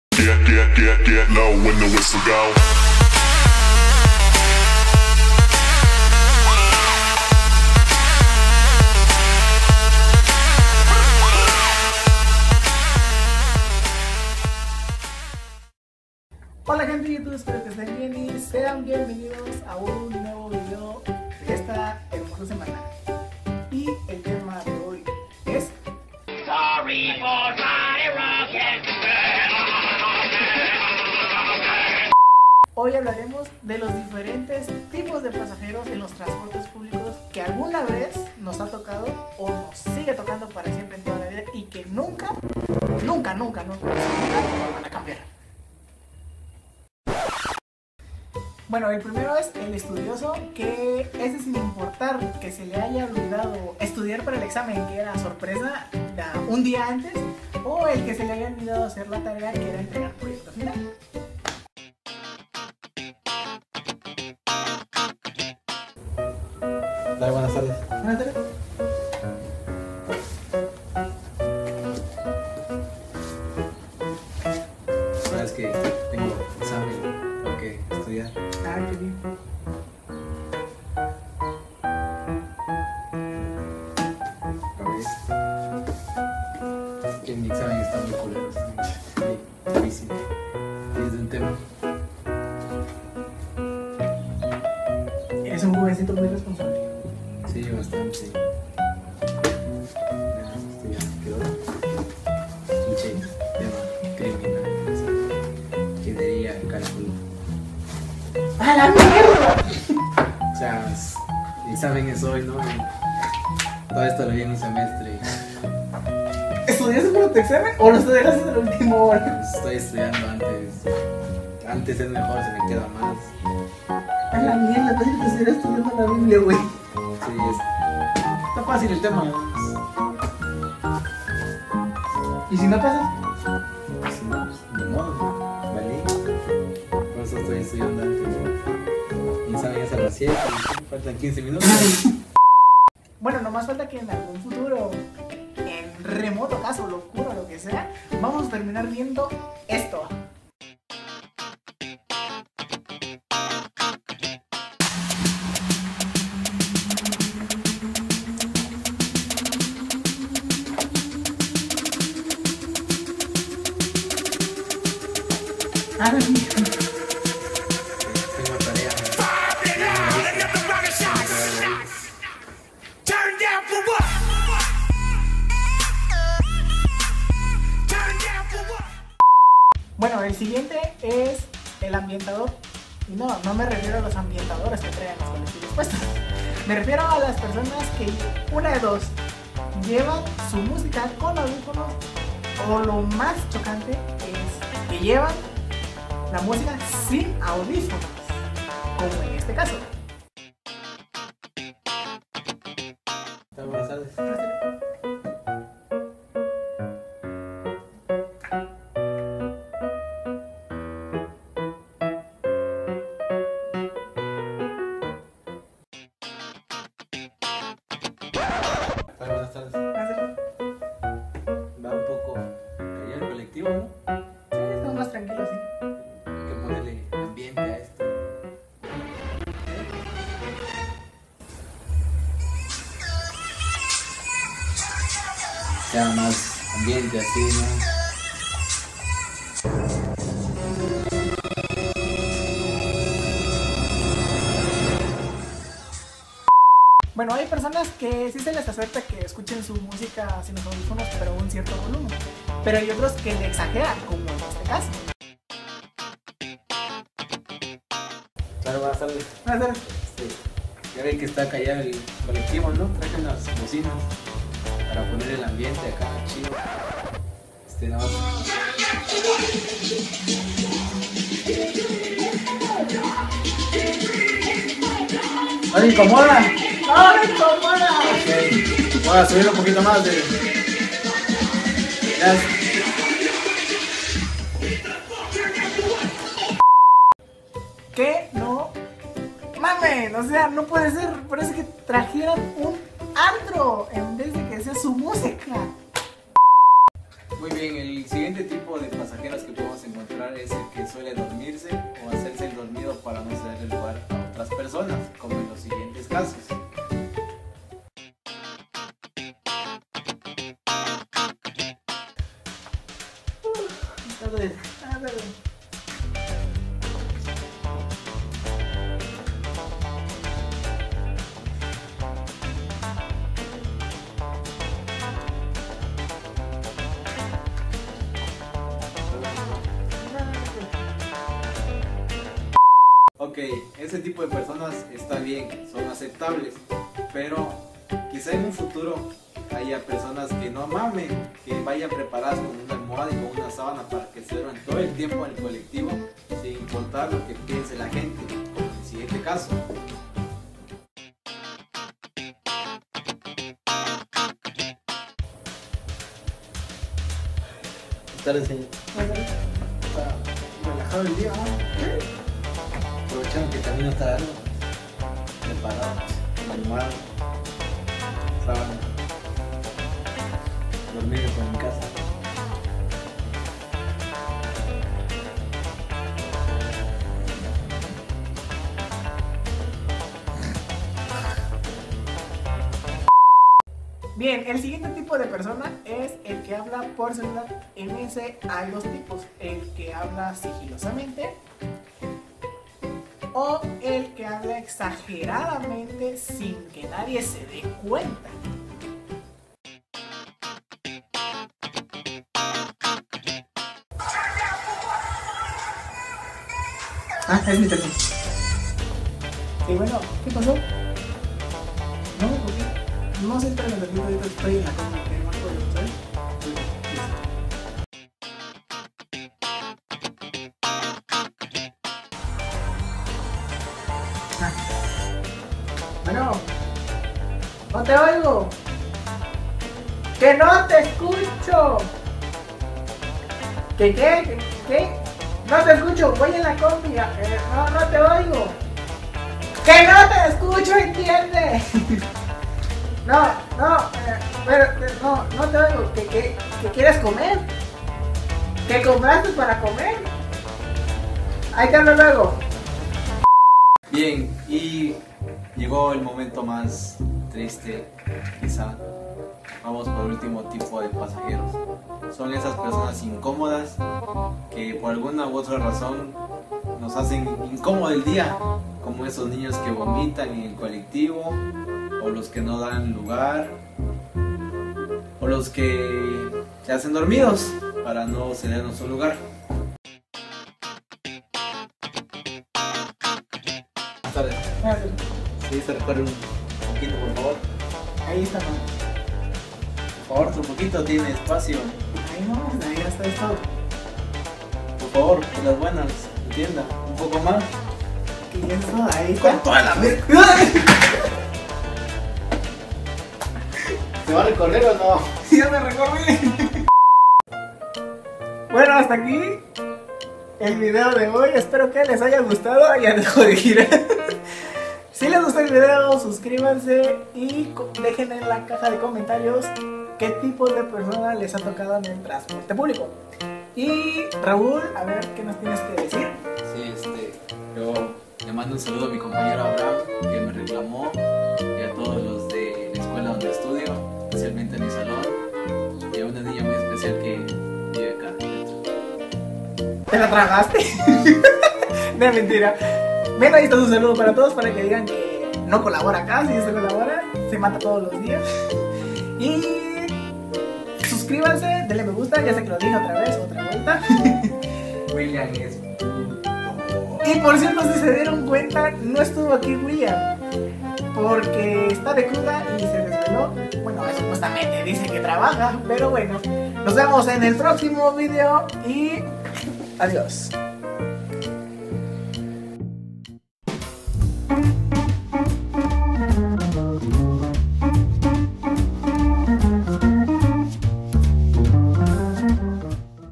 Vlog 04 - Tipos De Personas (Transporte Publico)
Espero que les guste este video sobre los tipos de personas que siempre lidiamos en los transportes publicos

Like si te a tocado a uno de los que mencionamos
Comenta alguno que no hayamos mencionado
Comparte para que los demas identifiquen a esas personitas